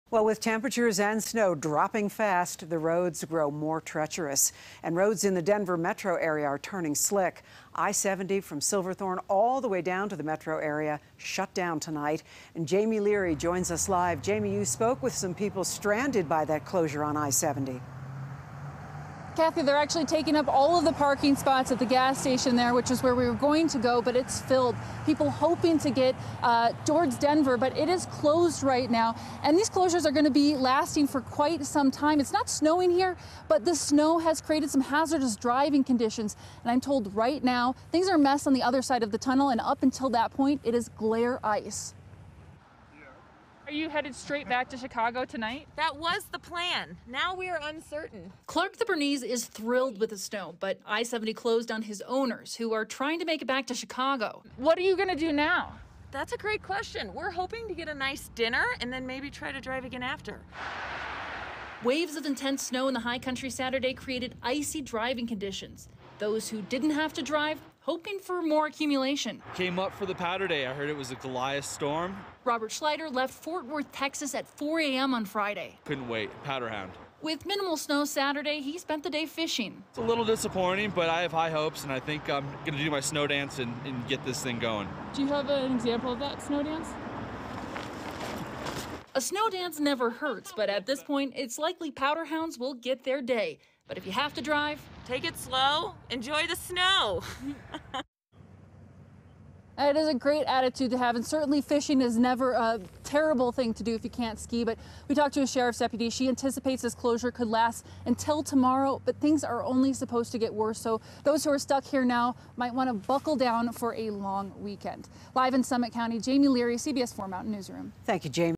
Colorado Snowstorm: I-70 Closed Between Morrison And Silverthorne
The Colorado State Patrol closed Interstate 70 between Morrison and Silverthorne Saturday night.